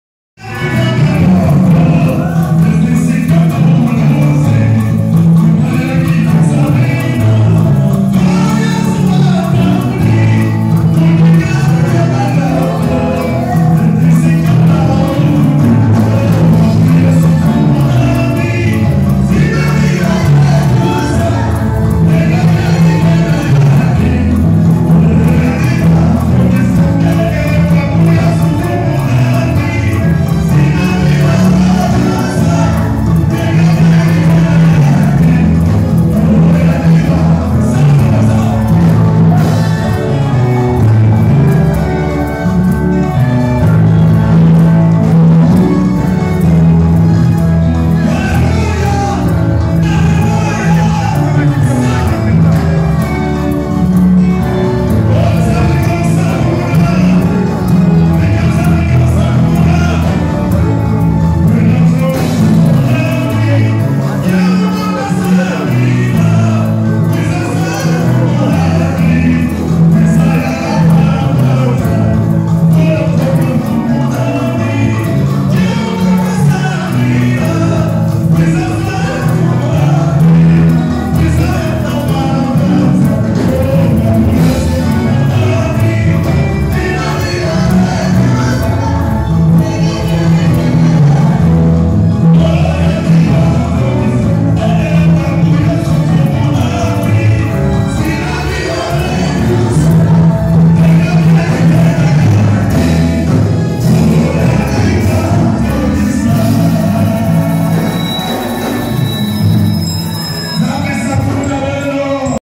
PAROISSE CEC KIAMVU / DIMANCHE, 20/01/019
EGLISE DU CHRIST AU CONGO
23è Communauté Evangélique du Congo
PAROISSE KIAMVU
Consistoire Matadi 1
MOMENT DE LOUANGE SHOW AVEC LE FRERE JOSE NZITA ET LES GROUPES INSTRUMENTAUX PAROISSE CEC KIAMVU :

= Groupe G.M.I LA PIERRE ANGULAIRE
= Groupe  G.I.A